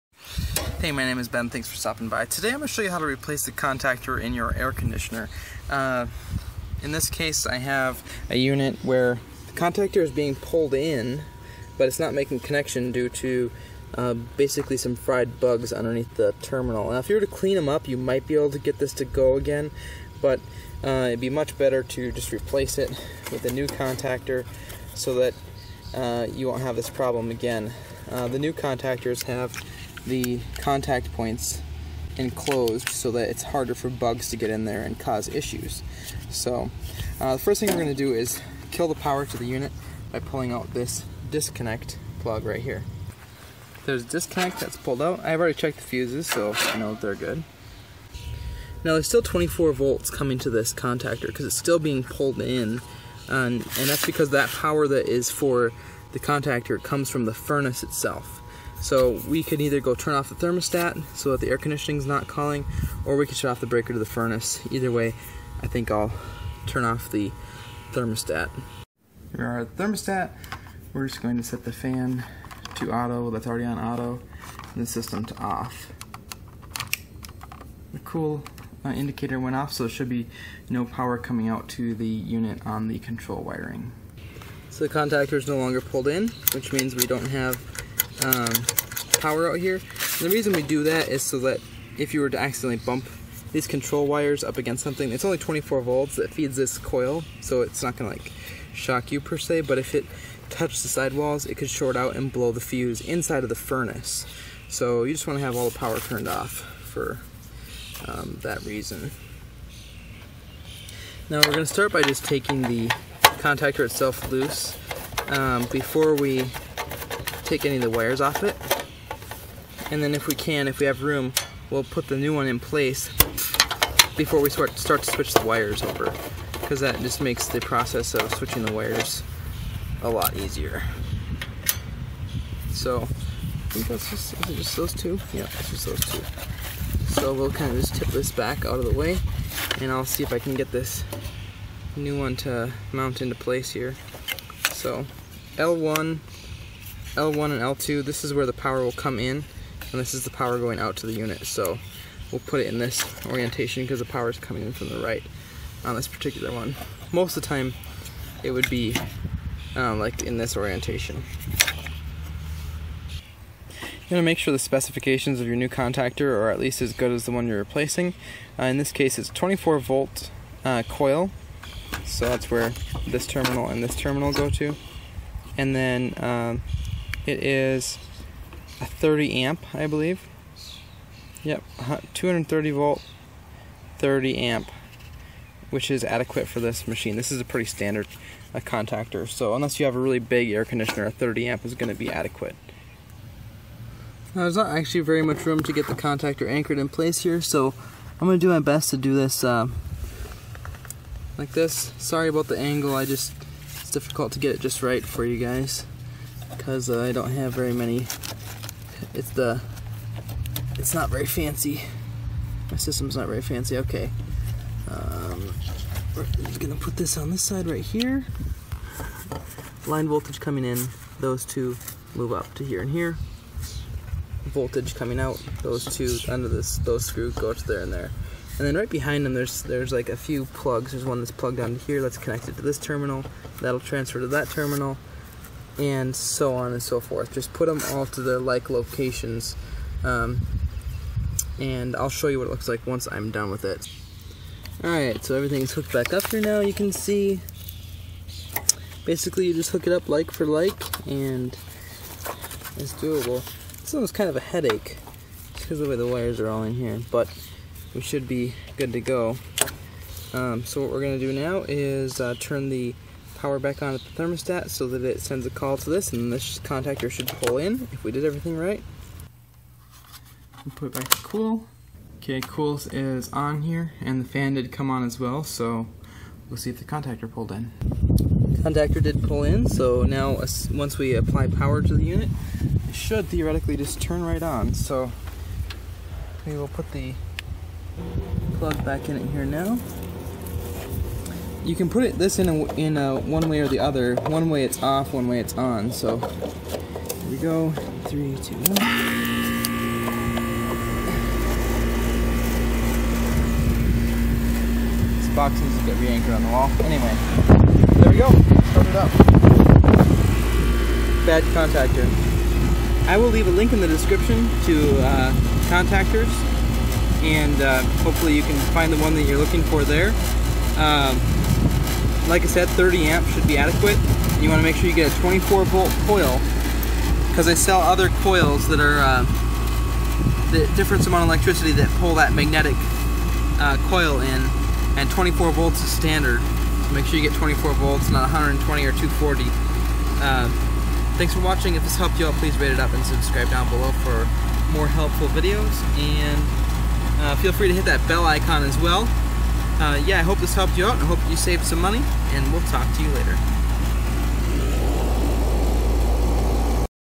How To Replace a Contactor in an Air Conditioner - AC Repair & Troubleshooting
24v Coil 40AMP Contactor
24v Coil 30AMP Contactor

Wiha Insulated Screwdriver Set
Tools We Recommend

Most common replacement contactor links above. You need to confirm basically three things before ordering a new contactor:
1 - Number of Poles (Most will be a 2 pole for standard 230v applications. Three pole would be for a three phase unit.)
2 - Coil Voltage (This is the power that supplies the electromagnet that pulls in the contactor. This is almost always 24 volts but there is a small chance it could be different.)
3 - AMPS (40A is the most common but yours could be larger. Be sure to get one at least as big or bigger than the amp rating of your current contactor.)

0:00 - How To Replace a Contactor Intro
0:15 - Repair Or Replace AC Contactor?
0:38 - Disconnect Power To Unit
0:48 - Disable 12V Call Power
1:50 - Remove Contactor
2:44 - New Contactor Specifications
3:33 - Transfer Connections To New Contactor
4:22 - Test New Contactor
5:40 - SUBSCRIBE!!!